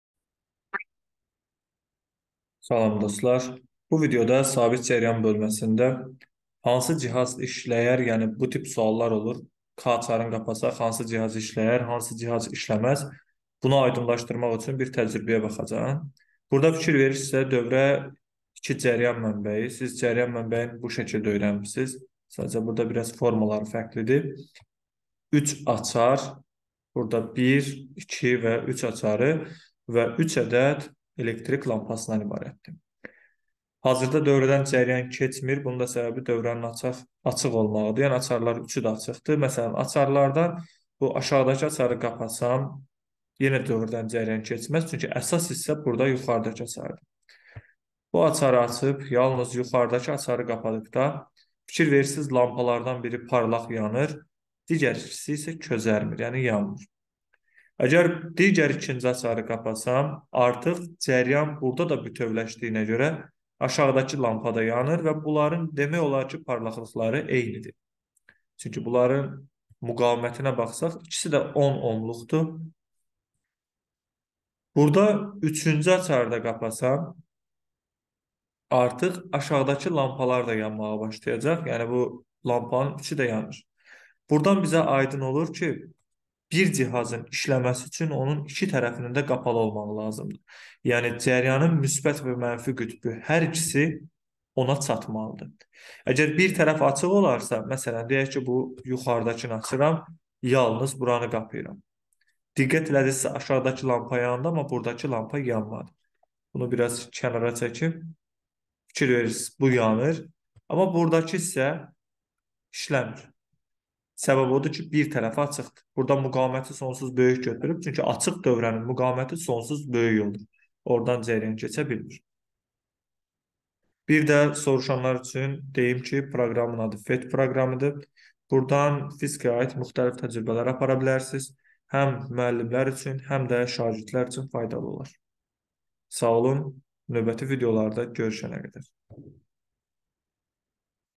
Sabit cərəyan eksperiment. Dövrə yığmaq və cihazların qoşulması.
Bu zaman kodlarından istifadə edərək sizə lazım olan bölməyə birbaşa keçid edə bilərsiniz.
00:00 – Giriş;
🔴Xoş seyrlər
👤Surət Xəlilov